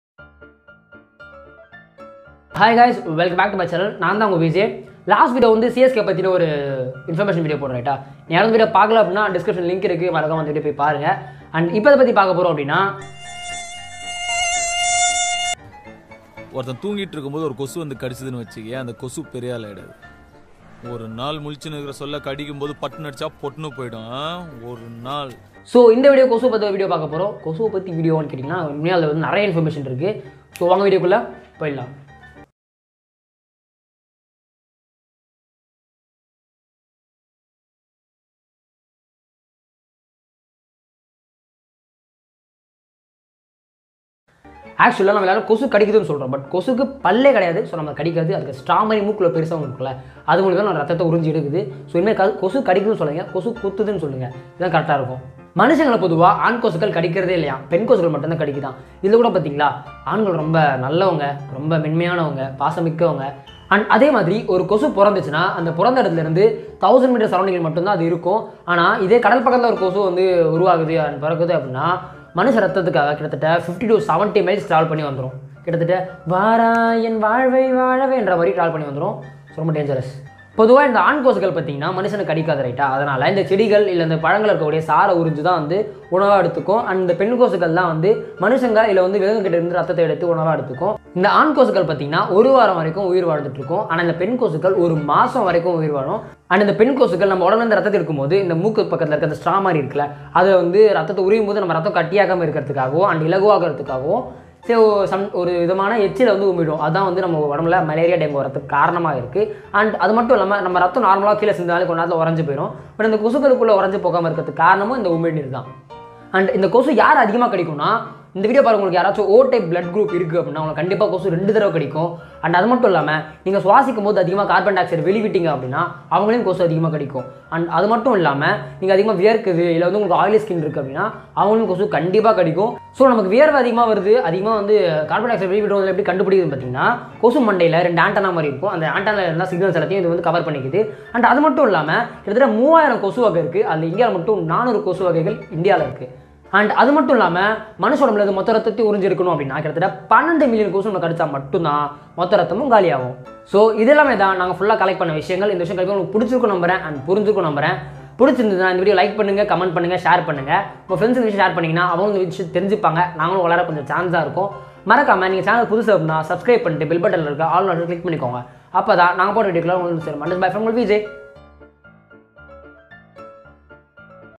கொசு தொல்லை தாங்க முடியல | Intresting information about MOSQUITO | Talk 2 VJ
This video about to MOSQUITO 😂 because its very irritating and annoying insects ever so we don't know about its normal behaviour so watch this video till the end ✌️

For any enquiries and improvements please contact our crew members 🙏❤️